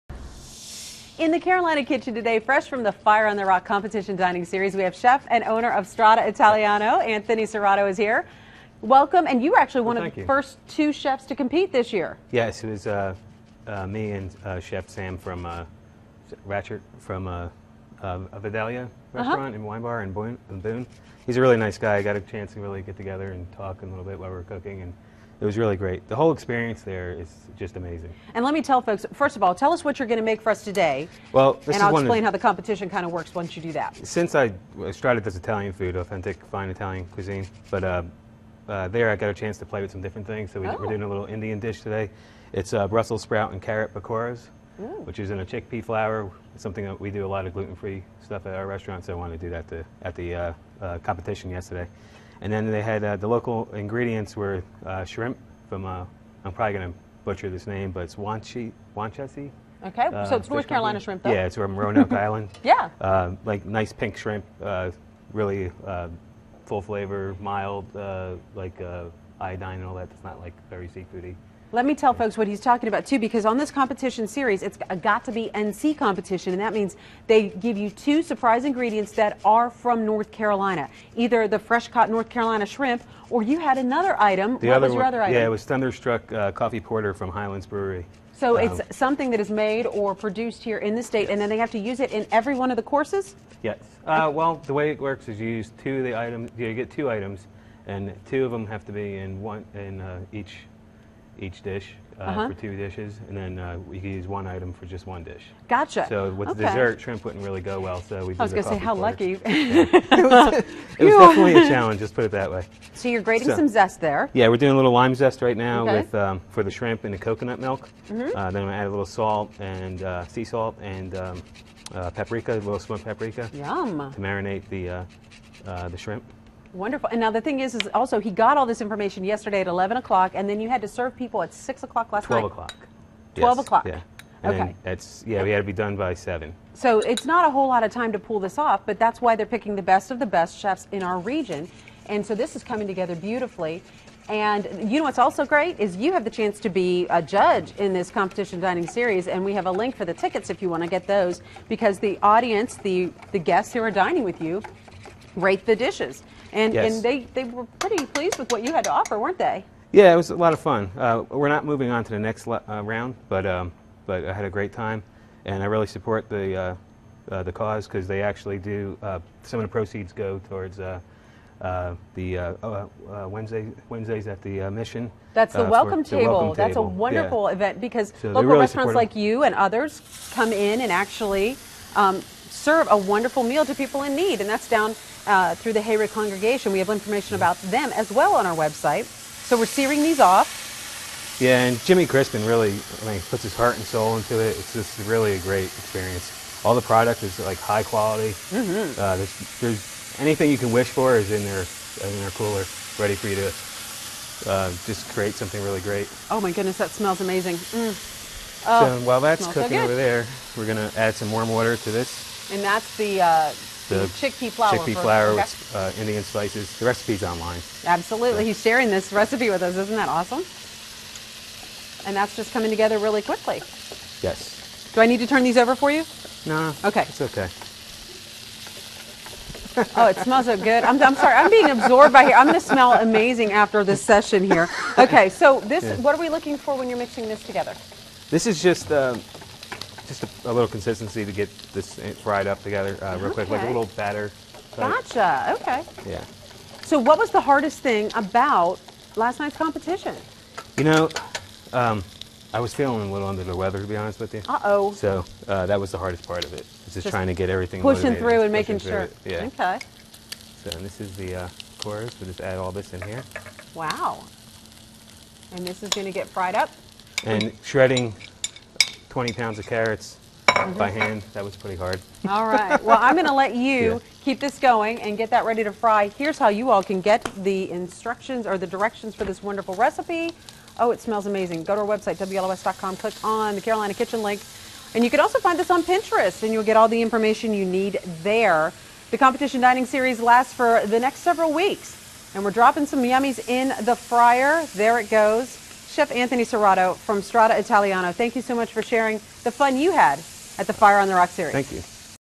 Fire on the Rock Competition Dining Series
Pakora Batter
by Strada Italiano Chef Anthony Cerrato

Ingredients
2 Cups chickpea flour
1/2 Cup chopped cilantro
1 1/2 TBs turmeric
2 TBS chopped chillies
1 TBS coriander
Salt and pepper to taste
1 heavy TBS chili powder
5 Cups warm water

Instructions
Combine everything but cilantro and chillies in bowl. Add warm water to dry ingredients and mix until smooth. Add cilantro and chillies, incorporate, then season to taste.

Ingredients
5 apples, diced
1 Cup dried cherries
Pinch ginger powder
1 whole nutmeg
1 Cup sherry vinegar
1 1/2 Cups water
1 1/2 Cups sugar
Salt to taste
1 stick cinnamon

Instructions
Combine all ingredients in a pot. Cook on medium heat until liquid is thick (syrup) and solids are softened. Remove cinnamon stick and serve warm.

Ingredients
1 bunch cilantro, chopped (with stems)
3 TBS Coconut milk
8 cloves garlic
Juice of 3 limes
1/4 Cup sugar
2 TBS salt
1 Cup water

Instructions
Combine sugar, lime and garlic in blender. Pulse until thick paste forms. Add cilantro, water and coconut milk and blend until smooth.

Ingredients
6 each NC Shrimp
1/2 Cup coconut puree
1/2 tsp sweet paprika
1 each lime zest

Instructions
Marinate shrimp in coconut puree, mix with sweet paprika and lime zest. Skewer three shrimp on each skewer and pan sear.